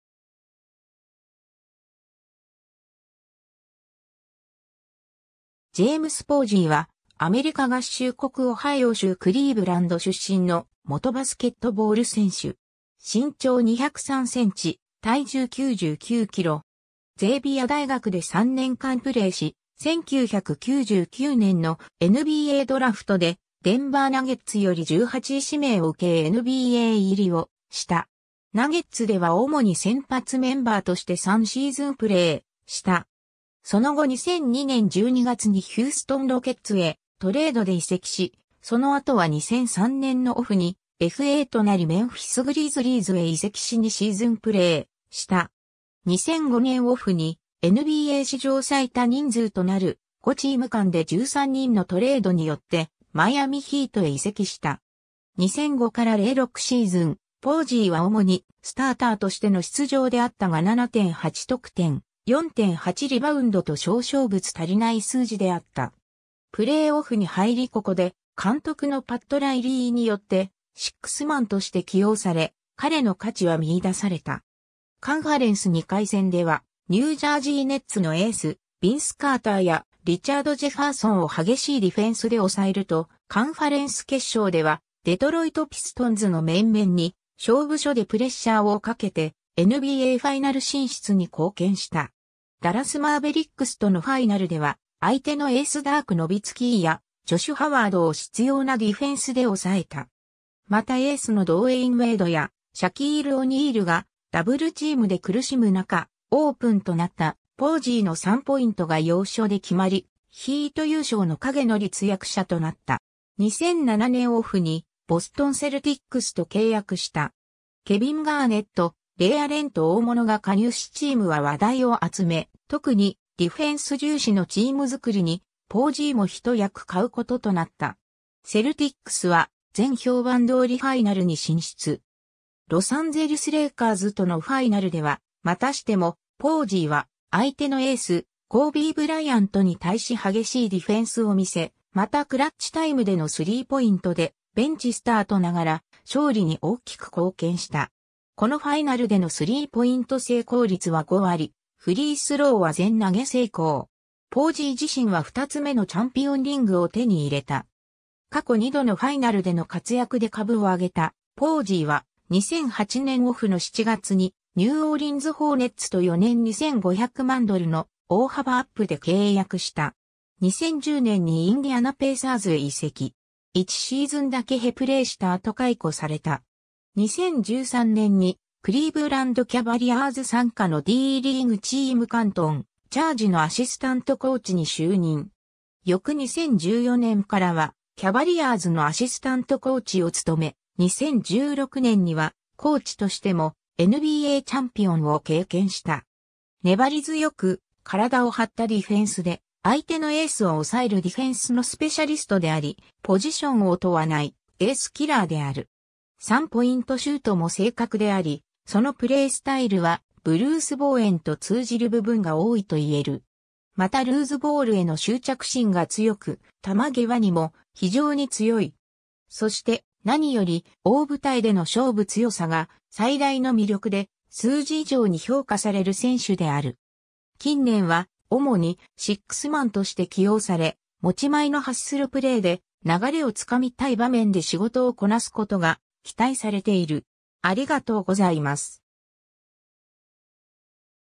ジェームズ・ポージー
クリーブランド・キャバリアーズ関連人物
ジェームス・ポージー（James Mikely Mantell Posey, Jr. 1977年1月13日 - ）は、アメリカ合衆国オハイオ州クリーブランド出身の元バスケットボール選手。
身長203センチ、体重99キロ。
 ゼイビア大学で3年間プレーし、1999年のNBAドラフトでデンバー・ナゲッツより18位指名を受けNBA入りをした。
 ナゲッツでは主に先発メンバーとして3シーズンプレーした。
その後2002年12月にヒューストン・ロケッツへトレードで移籍し、その後は2003年のオフにFAとなりメンフィス・グリズリーズへ移籍し2シーズンプレーした。
 2005年オフに、NBA史上最多人数となる、5チーム間で13人のトレードによってマイアミ・ヒートへ移籍した。
2005-06シーズン、ポージーは主にスターターとしての出場であったが7.8得点、4.8リバウンドと少々物足りない数字であった。
プレーオフに入りここで監督のパット・ライリーによってシックスマンとして起用され、彼の価値は見出された。
カンファレンス2回戦ではニュージャージー・ネッツのエース、ヴィンス・カーターやリチャード・ジェファーソンを激しいディフェンスで抑えると、カンファレンス決勝ではデトロイト・ピストンズの面々に、勝負所でプレッシャーをかけて、NBAファイナル進出に貢献した。
 ダラス・マーベリックスとのファイナルでは相手のエースダーク・ノビツキーやジョシュ・ハワードを執拗なディフェンスで抑えた。
またエースのドウェイン・ウェイドやシャキール・オニールがダブルチームで苦しむ中、オープンとなったポージーの３ポイントが要所で決まり、ヒート優勝の影の立役者となった。
 2007年オフに、ボストン・セルティックスと契約した。
ケビン・ガーネット、レイ・アレンと大物が加入しチームは話題を集め、特にディフェンス重視のチーム作りにポージーも一役買う事となった。
セルティックスは前評判通りファイナルに進出。
ロサンゼルス・レイカーズとのファイナルでは、またしてもポージーは相手のエース、コービー・ブライアントに対し激しいディフェンスを見せ、またクラッチタイムでのスリーポイントで、ベンチスタートながら勝利に大きく貢献した。
このファイナルでのスリーポイント成功率は5割、フリースローは全投成功。
ポージー自身は2つ目のチャンピオンリングを手に入れた。
 過去2度のファイナルでの活躍で株を上げたポージーは、2008年オフの7月にニューオーリンズ・ホーネッツと4年2500万ドルの大幅アップで契約した。
 2010年にインディアナ・ペイサーズへ移籍。
1シーズンだけプレーした後解雇された。
 2013年にクリーブランド・キャバリアーズ傘下のDリーグチームカントン・チャージのアシスタントコーチに就任。
翌2014年からはキャバリアーズのアシスタントコーチを務め、2016年にはコーチとしてもNBAチャンピオンを経験した。
 粘り強く体を張ったディフェンスで相手のエースを抑えるディフェンスのスペシャリストであり、ポジションを問わないエースキラーである。
3ポイントシュートも正確であり、そのプレースタイルはブルース・ボウエンと通じる部分が多いと言える。
またルーズボールへの執着心が強く、玉ぎわにも非常に強い。
そして何より大舞台での勝負強さが最大の魅力で、数字以上に評価される選手である。
近年は、主にシックスマンとして起用され、持ち前のハッスルプレーで、流れをつかみたい場面で仕事をこなす事が期待されている。